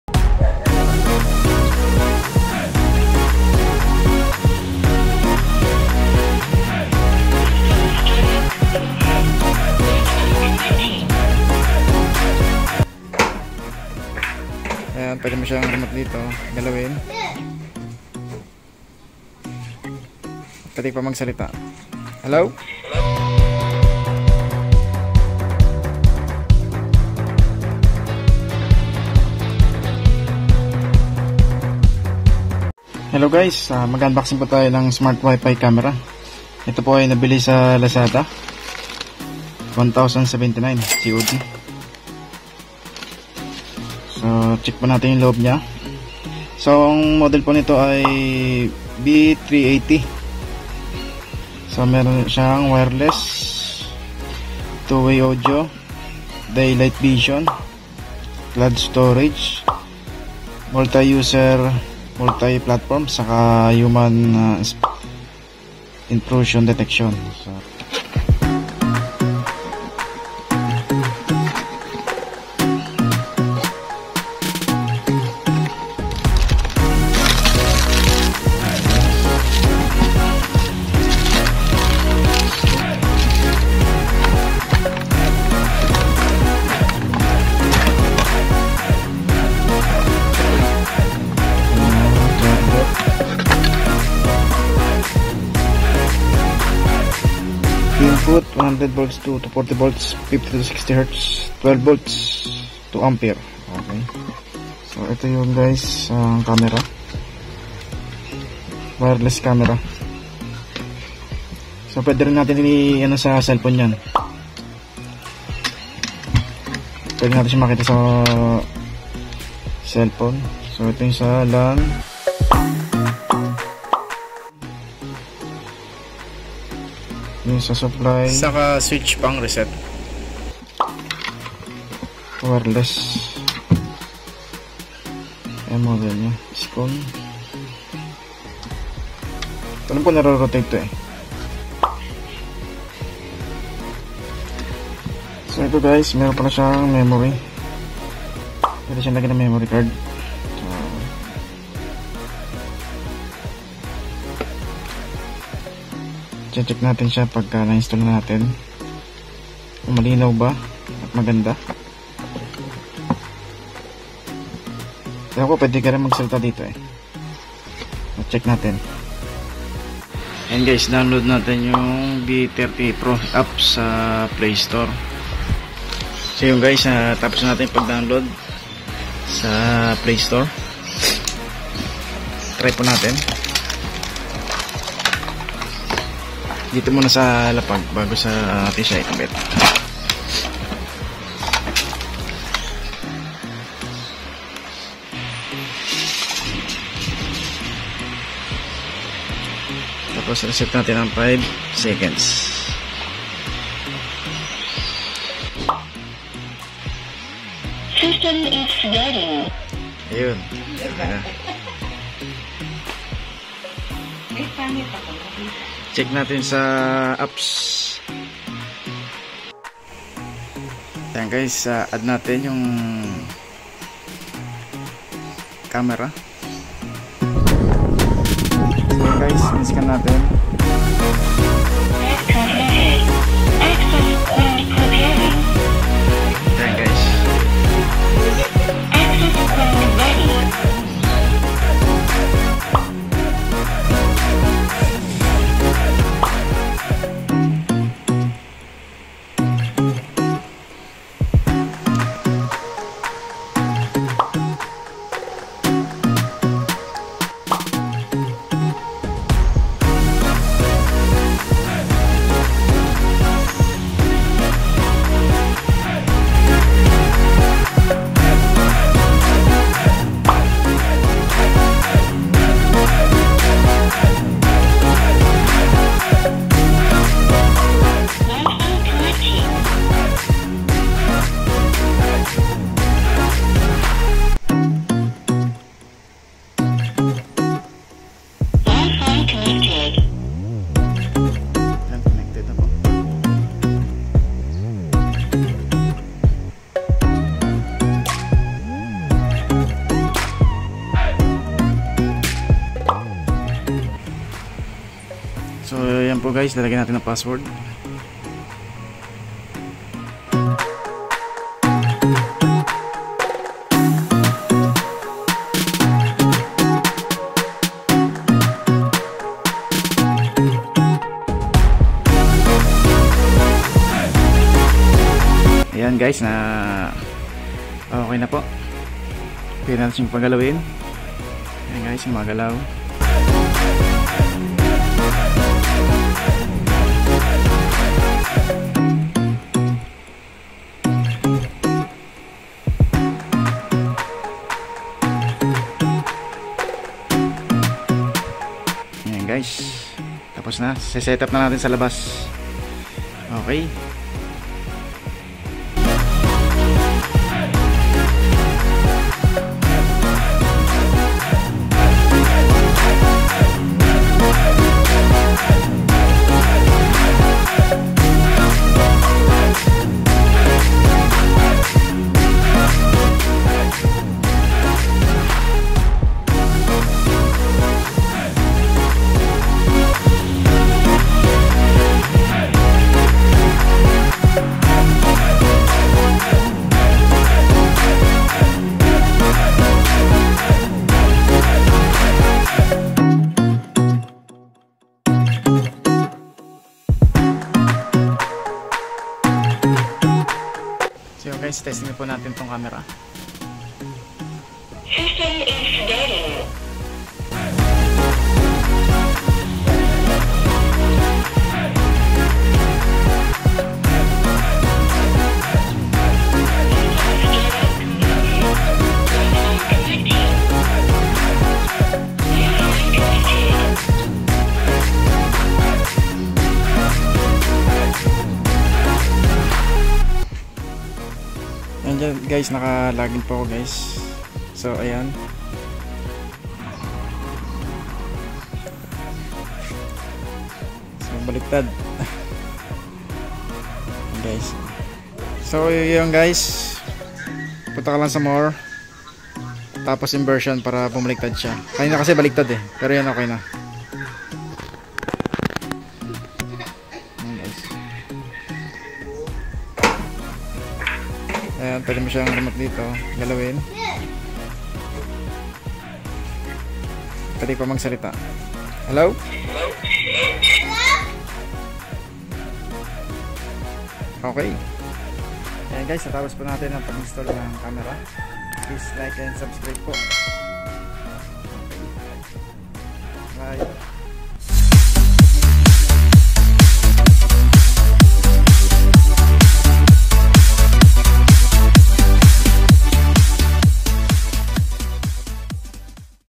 HOW TO SET UP SKONE V380 PRO APPS AND UNBOXING | MURANG CCTV V380PRO HD | JAYSON PERALTA TECH
In this video, i will share to you how to set up and installation of skone v380 pro and apps in google play store.
the skone smart camera price is 1079 pesos in lazada.ph store.